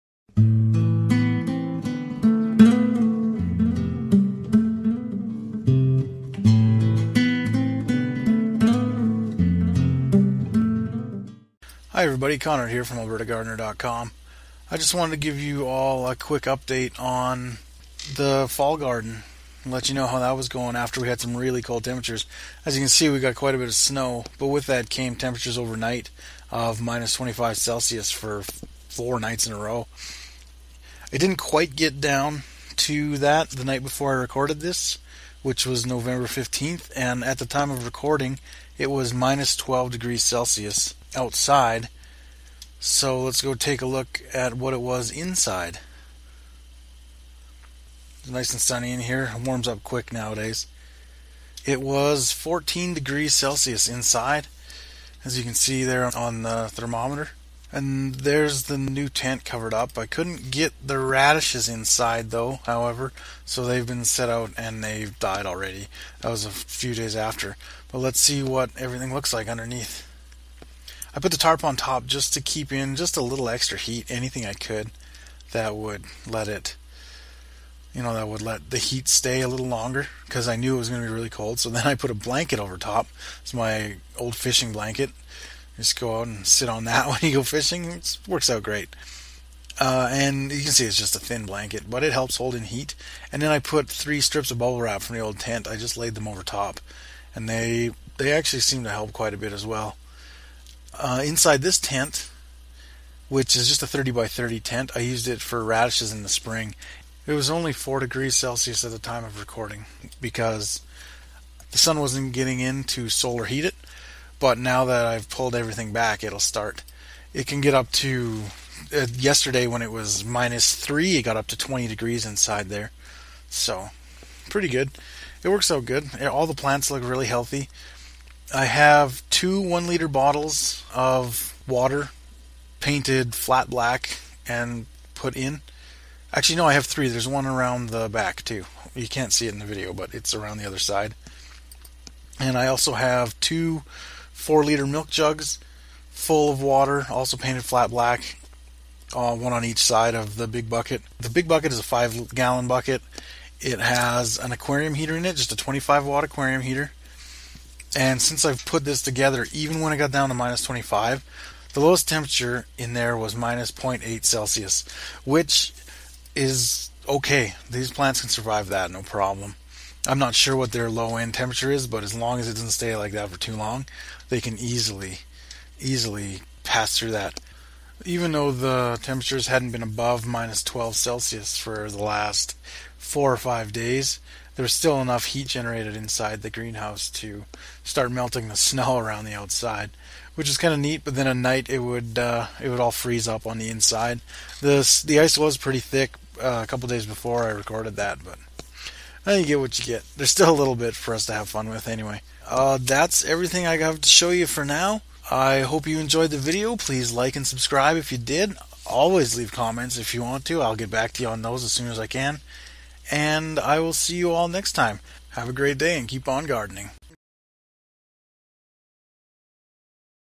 Fall Garden Update Nov 15, 2014
Another look at the fall garden to see how it is doing after some bitterly cold November nights.